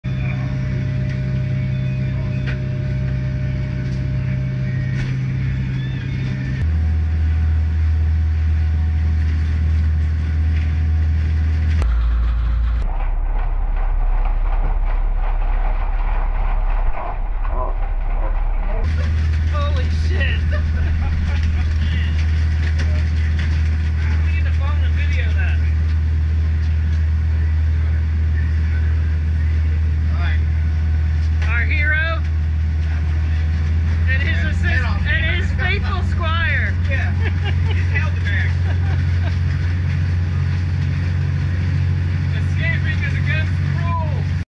Lobster bag dump - Jupiter Fl July 26, 2018
Excellent haul - Mini Season day 2  with Pat, Sam, Tony, Anthony, Jef, Jim & JB